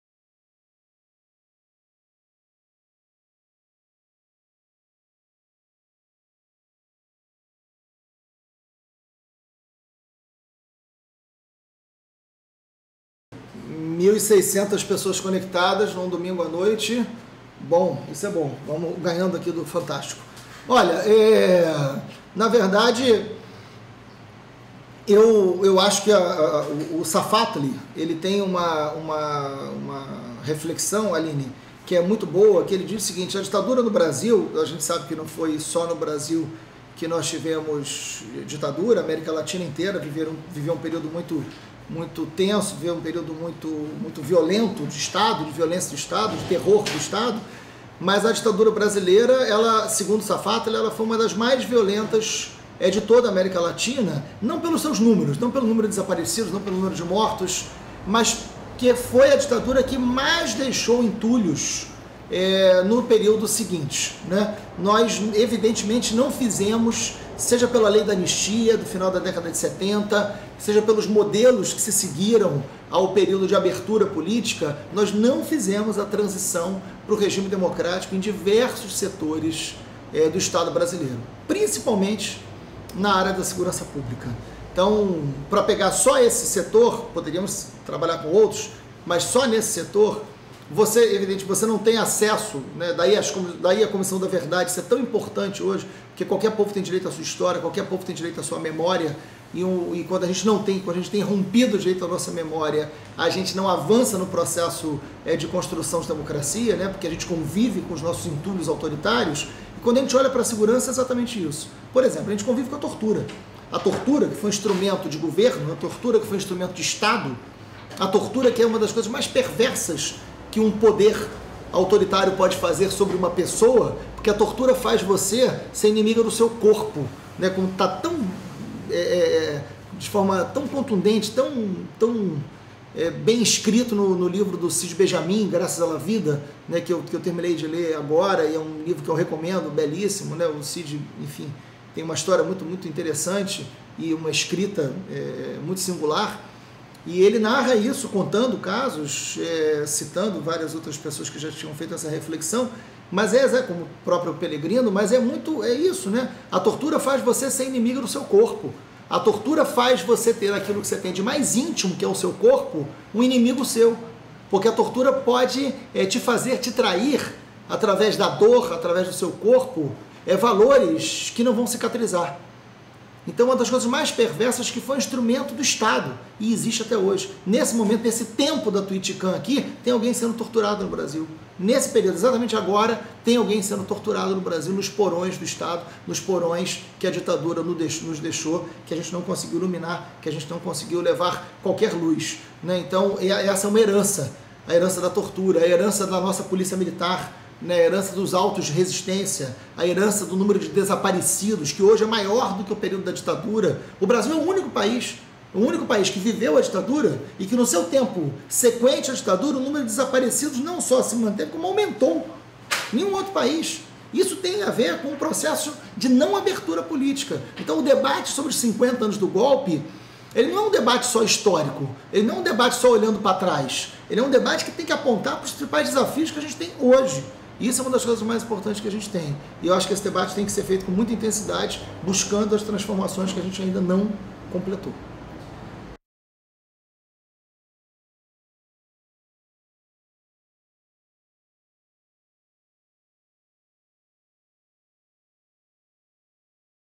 Twitcam - 30/03: 50 anos do Golpe Militar
Marcelo Freixo comenta sobre os 50 anos do Golpe Militar, na Twitcam do dia 30/03/14.